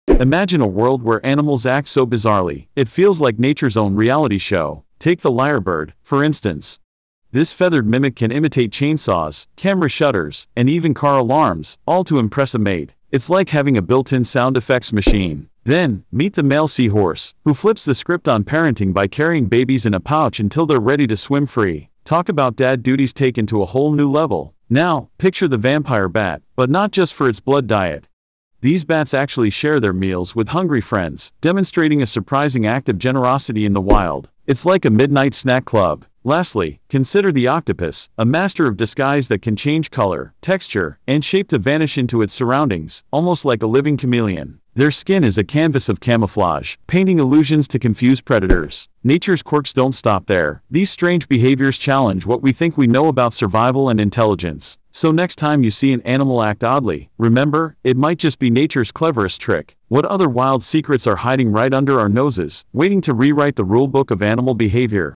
5 Mind-Blowing Animal Behaviors That Defy Nature’s Rules!
Discover the wildest animal behaviors you've never seen before! From the lyrebird’s incredible sound mimicry to the male seahorse’s unique parenting, the generous vampire bat, and the masterful camouflage of the octopus—these fascinating creatures prove nature is full of surprises. Watch now to uncover these amazing survival tricks and rethink what you know about animal intelligence and behavior!